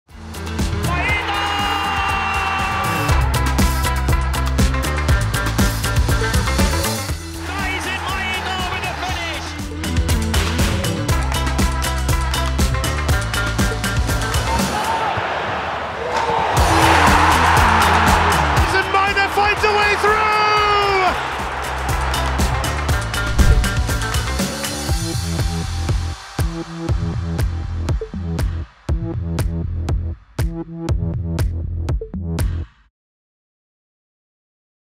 Daizen Maeda commits future to Celtic with a new four-year deal!
We are very happy to announce that our Treble-Winning winger, Daizen Maeda has signed a new four-year deal with the Club! 🇯🇵💚